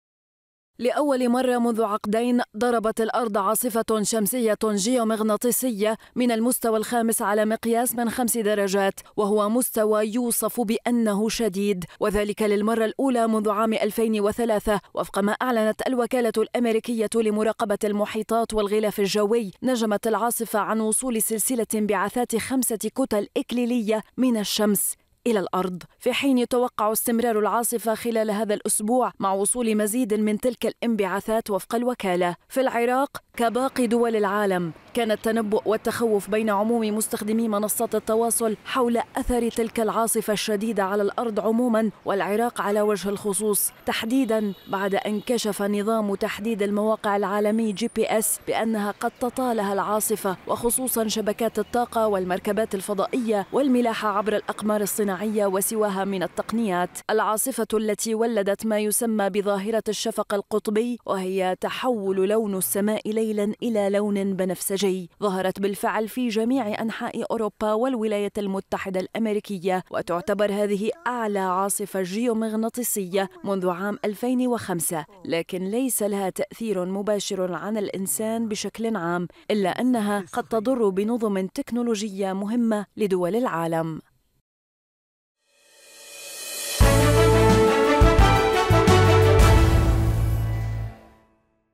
الاشد منذ 2015 .. عاصفة شمسية تتجه نحو العراق | تقرير
الاشد منذ 2015 .. عاصفة شمسية تتجه نحو العراق
يتندر عراقيون بتداعيات العاصفة الشمية على البلاد بعد تأكيد خبراء أن العاصفة تعتبر الأشد منذ 2015 وتهدد نظما تكنولوجية مهمة في قطاعي الاتصالات والطاقة حول العالم

موقع قناة التغيير الفضائية  الالكتروني